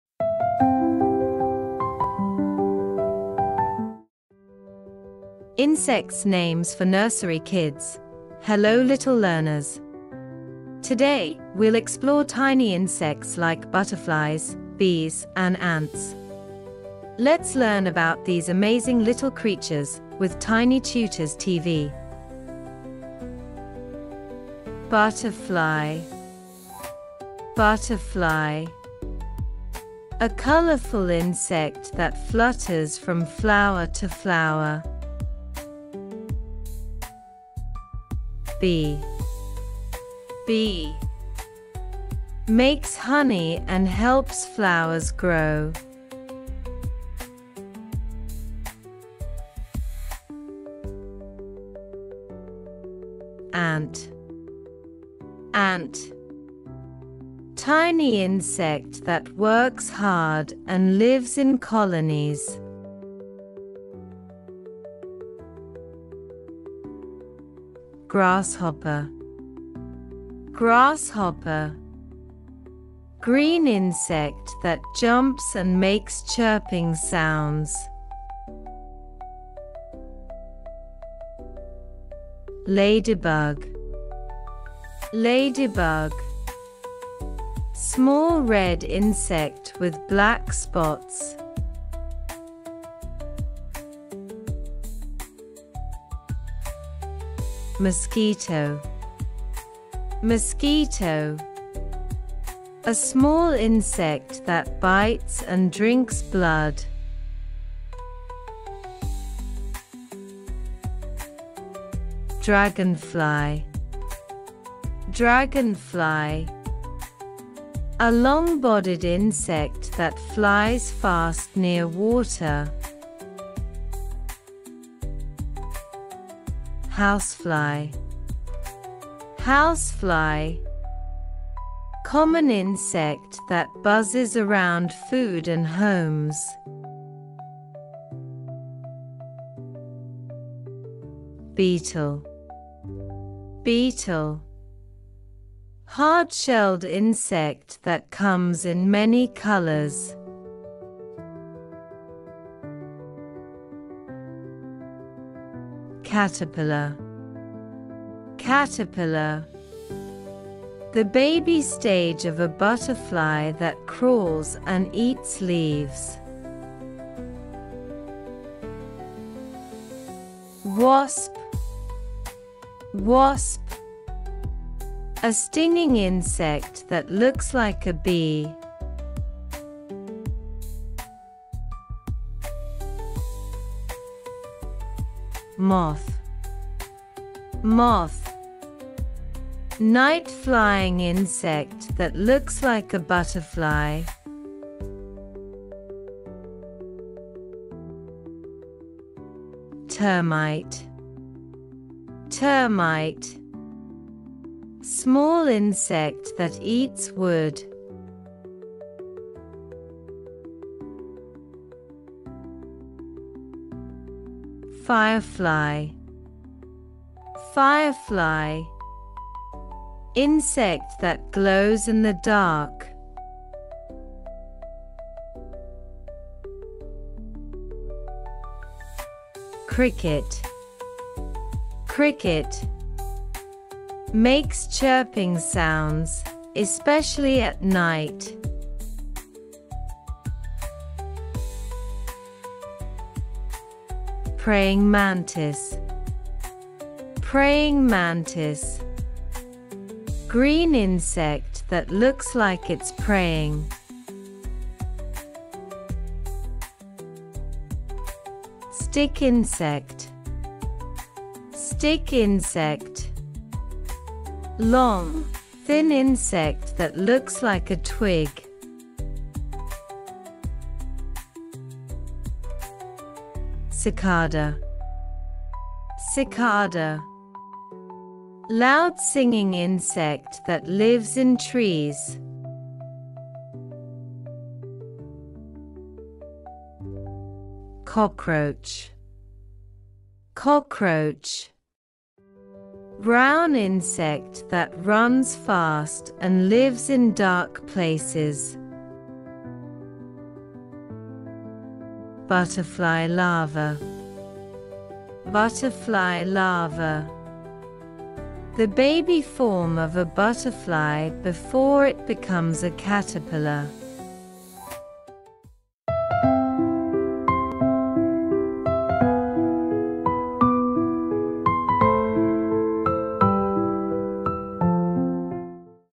Insects Around Us | Fun Insect Lesson for Nursery Kids | Tiny Tutors TV
Hello little learners! 🌟
Insects are tiny creatures that live all around us. In this fun and colorful lesson, we will learn about 20 amazing insects — from butterflies and bees to ants, grasshoppers, and ladybugs. 🐝🦋🐞

Insects We’ll Learn:

Butterfly 🦋: A colorful insect that flutters from flower to flower.
Bee 🐝: Makes honey and helps flowers grow.
Ant 🐜: Tiny insect that works hard and lives in colonies.
Grasshopper 🦗: Green insect that jumps and makes chirping sounds.
Ladybug 🐞: Small red insect with black spots.
Mosquito 🦟: A small insect that bites and drinks blood.
Dragonfly 🐉: A long-bodied insect that flies fast near water.
Housefly 🪰: Common insect that buzzes around food and homes.
Beetle 🪲: Hard-shelled insect that comes in many colors.
Caterpillar 🐛: The baby stage of a butterfly that crawls and eats leaves.
Wasp 🐝: A stinging insect that looks like a bee.
Moth 🦋: Night-flying insect that looks like a butterfly.
Termite 🐜: Small insect that eats wood.
Firefly ✨: Insect that glows in the dark.
Cricket 🦗: Makes chirping sounds, especially at night.
Praying Mantis 🦗: Green insect that looks like it’s praying.
Stick Insect 🪱: Long, thin insect that looks like a twig.
Cicada 🦗: Loud-singing insect that lives in trees.
Cockroach 🪳: Brown insect that runs fast and lives in dark places.
Butterfly Larva 🐛: The baby form of a butterfly before it becomes a caterpillar.

Let’s explore the tiny, wonderful world of insects together in this fun and easy lesson! 🌈